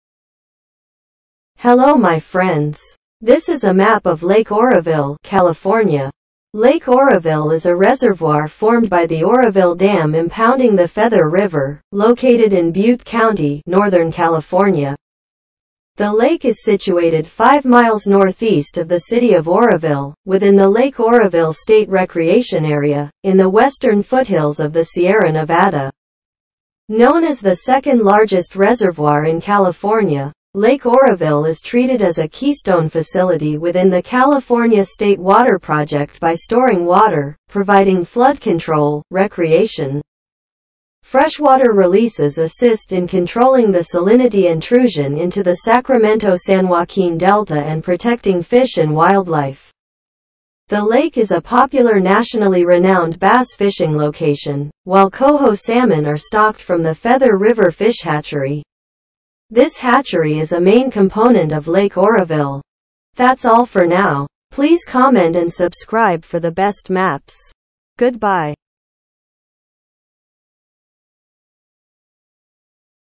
MAP OF LAKE OROVILLE CALIFORNIA
hello my friends. this is a map of Lake Oroville , California. Lake Oroville is a reservoir formed by the Oroville Dam impounding the Feather River, located in Butte County, northern California. The lake is situated 5 miles northeast of the city of Oroville, within the Lake Oroville State Recreation Area, in the western foothills of the Sierra Nevada. Known as the second-largest reservoir in California, Lake Oroville is treated as a keystone facility within the California State Water Project by storing water, providing flood control, recreation, freshwater releases assist in controlling the salinity intrusion into the Sacramento-San Joaquin Delta and protecting fish and wildlife.

The lake is a popular nationally renowned bass fishing location, while coho salmon are stocked from the Feather River Fish Hatchery.